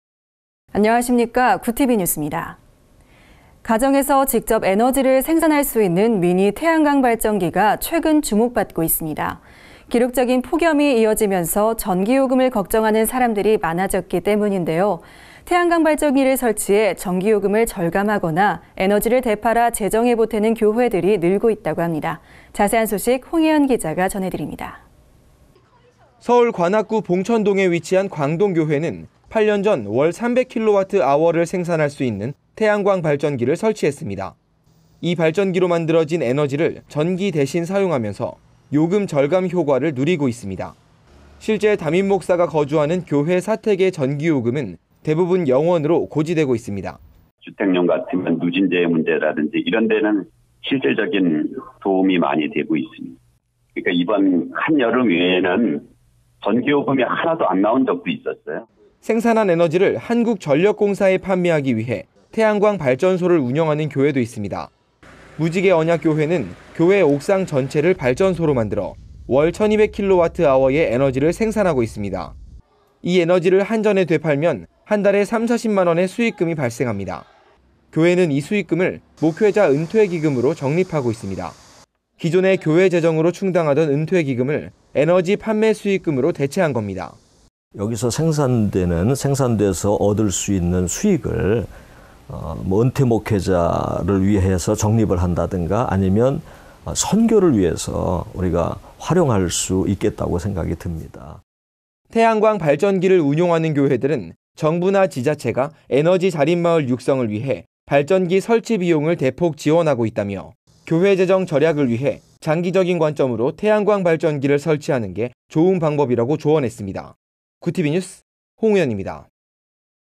[Goodtv News 20160824] 누진제 걱정…'태양광발전'으로 해결
안녕하십니까, GOODTV 뉴습니다.
가정에서 직접 에너지를 생산할 수 있는 '미니 태양광 발전기'가 최근 주목받고 있습니다. 기록적인 폭염이 이어지면서 전기요금을 걱정하는 사람들이 많아졌기 때문인데요. 태양광 발전기를 설치해 전기 요금을 절감하거나, 에너지를 되팔아 재정에 보태는 교회들이 늘고 있다고 합니다. 자세한 소식 홍의현 기자가 전해드립니다.

서울 관악구 봉천동에 위치한 광동교회는 8년 전 '월 300kWh'를 생산할 수 있는 태양광 발전기를 설치했습니다.

이 발전기로 만들어진 에너지를 전기 대신 사용하면서 요금 절감 효과를 누리고 있습니다.

실제 담임목사가 거주하는 교회 사택의 전기요금은 대부분 0원으로 고지되고 있습니다.

(전화 인터뷰: 방영철 목사 / 광동교회)

생산한 에너지를 한국전력공사에 판매하기 위해 '태양광 발전소'를 운영하는 교회도 있습니다.

무지개언약교회는 교회 옥상 전체를 발전소로 만들어 월 1,200kWh의 에너지를 생산하고 있습니다. 이 에너지를 한전에 되팔면 한 달에 30~40만 원의 수익금이 발생합니다.

교회는 이 수익금을 '목회자 은퇴기금'으로 적립하고 있습니다. 기존에 교회 재정으로 충당하던 은퇴기금을 에너지 판매 수익금으로 대체한 겁니다.

(인터뷰: 최효석 목사 / 무지개언약교회)

태양광 발전기를 운용하는 교회들은 "정부나 지자체가 에너지 자립 마을 육성을 위해 발전기 설치 비용을 대폭 지원하고 있다"며 "교회 재정 절약을 위해 장기적인 관점으로 태양광 발전기를 설치하는 게 좋은 방법"이라고 조언했습니다.

GOODTV NEWS 홍의현입니다.